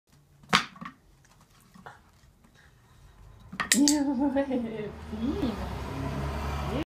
Geschicktes Schipperke 3
Hund zieht Wurfringe vom Stab des Ringwurfspiels